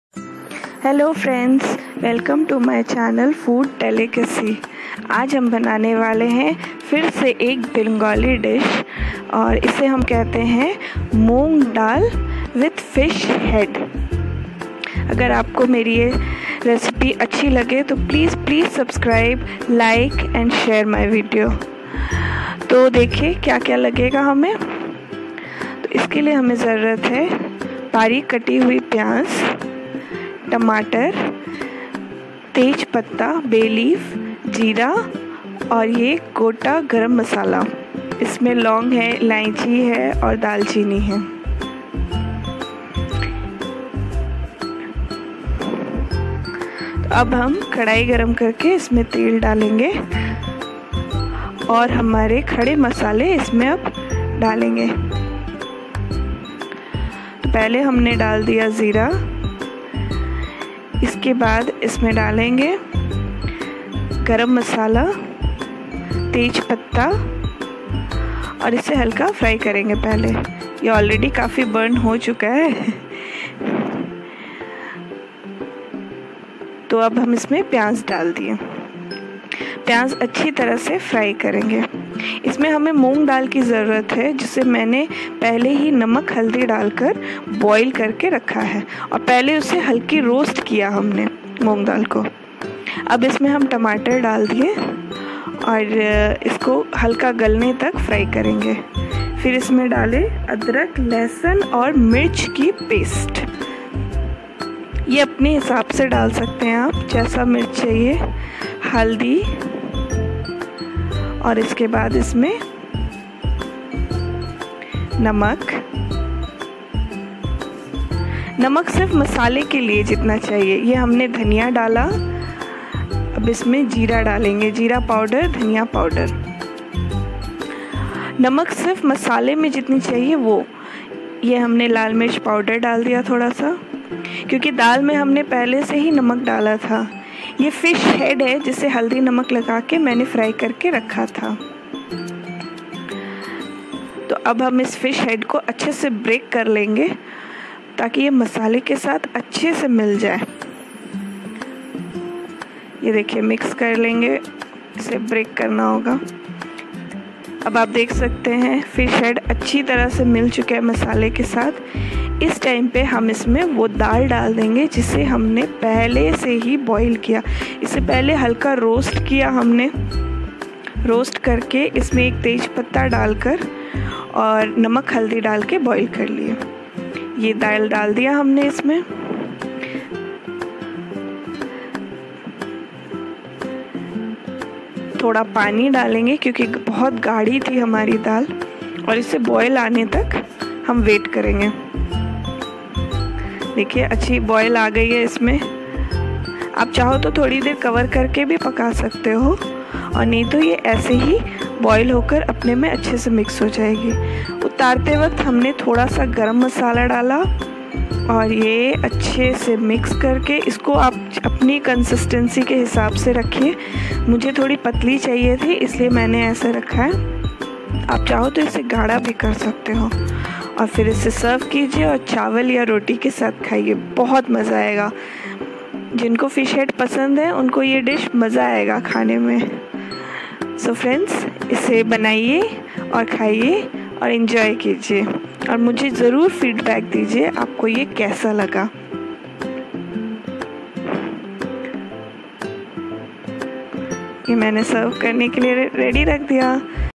macher maatha diye moong dal || fish head in moong daal || मछली का सर के साथ मूंग की दाल
hello friends
i am here again with a yet another bengali dish called machher matha diye moong daal/ fish head in moong daal
vry tasty and amazing dish to try.
so try it out and let me know your experience in the comment box.
stay blessed
stay happy